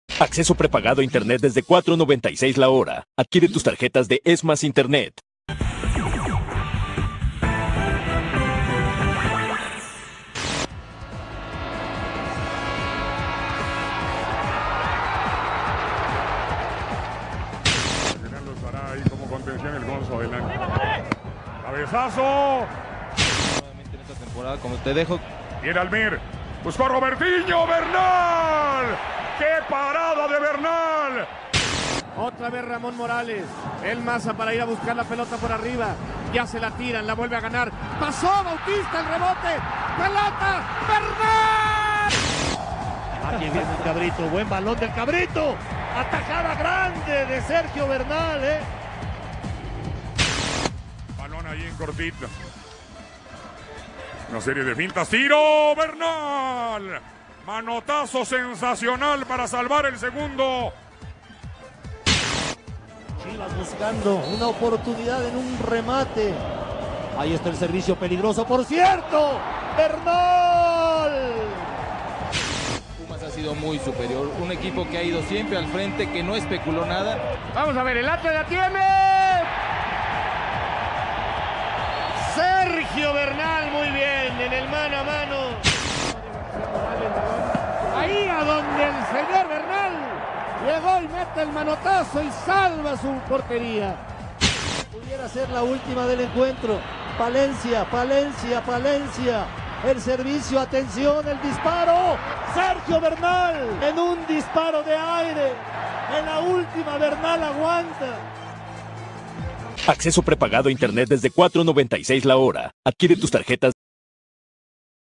SERGIO BERNAL PORTERO PUMAS DE LA UNAM BICAMPEON 2004
A short tribute to our goalkeeper, Sergio Bernal. The best goalkeeper in 2004 and one of the most important pieces to won both cups that year.

For a world without Amierdicanistas

|O|R|G|U|L|L|O||A|Z|U|L||Y||O|R|O|